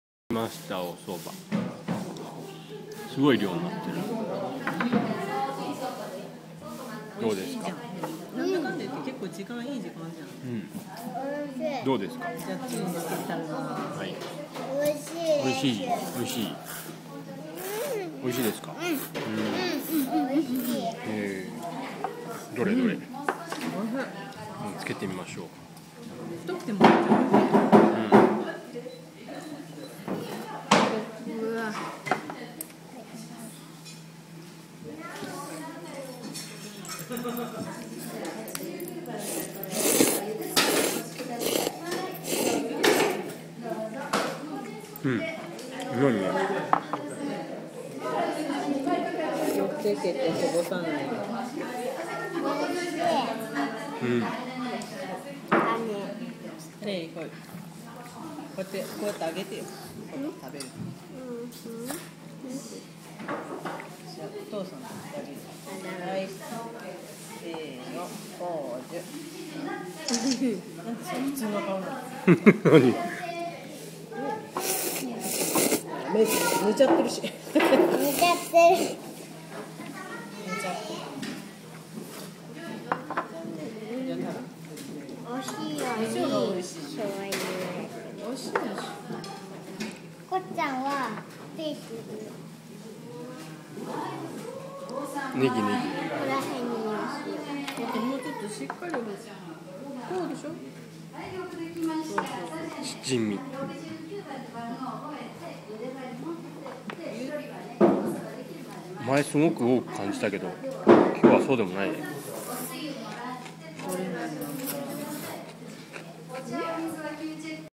2011年・夏 そば作り体験（10/10）
さて！さてさてさてさて・・・茹で上がりましたよ～＾＾
まず、自分達で粉を混ぜ合わせて、こねて、伸ばして、切って出来上がったのを
どんな感じか、チェックするのに、勧められたのは、まず、
１．何もつけずにそばだけを食す・・・
２．塩を一振りして食す・・・
３．そしてそばつゆにつけて食す・・・
４．ネギやワサビなどの薬味も入れて食す・・・
いやー、かなり旨かった！！触感が軽く、太さが均一ではなくても、大満足でした＾＾